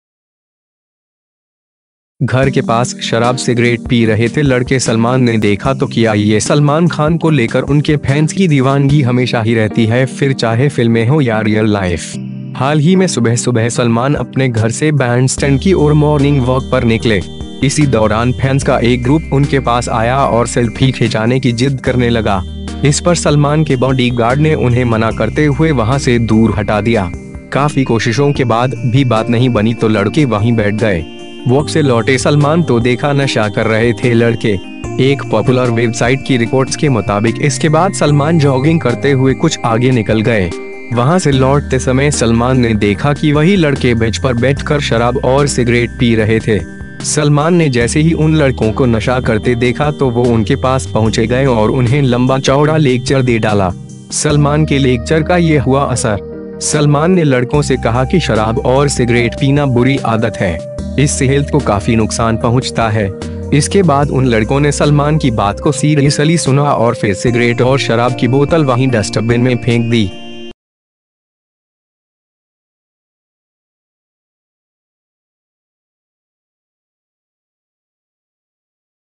घर के पास शराब सिगरेट पी रहे थे लड़के, सलमान ने देखा तो किया ये
सलमान खान को लेकर उनके फैंस की दीवानगी हमेशा ही रहती है, फिर चाहे फिल्में हों या रियल लाइफ। हाल ही में सुबह-सुबह सलमान अपने घर से बैंड स्टैंड की ओर मॉर्निंग वॉक पर निकले। इसी दौरान फैंस का एक ग्रुप उनके पास आया और सेल्फी खिंचाने की जिद करने लगा। इस पर सलमान के बॉडीगार्ड ने उन्हें मना करते हुए वहां से दूर हटा दिया। काफी कोशिशों के बाद भी बात नहीं बनी तो लड़के वहीं बैठ गए। वॉक से लौटे सलमान तो देखा नशा कर रहे थे लड़के...

- एक पॉपुलर वेबसाइट की रिपोर्ट्स के मुताबिक, इसके बाद सलमान जॉगिंग करते हुए कुछ आगे निकल गए। वहां से लौटते समय सलमान ने देखा कि वही लड़के बेंच पर बैठकर शराब और सिगरेट पी रहे थे।
- सलमान ने जैसे ही उन लड़कों को नशा करते देखा तो वो उनके पास पहुंचे गए और उन्हें लंबा-चौड़ा लेक्चर दे डाला।
सलमान के लेक्चर का ये हुआ असर...
- सलमान ने लड़कों से कहा कि शराब और सिगरेट पीना बुरी आदत है। इससे हेल्थ को काफी नुकसान पहुंचता है।
- इसके बाद उन लड़कों ने सलमान की बात को सीरियसली सुना और फिर सिगरेट और शराब की बोतल वहीं डस्टबिन में फेंक दी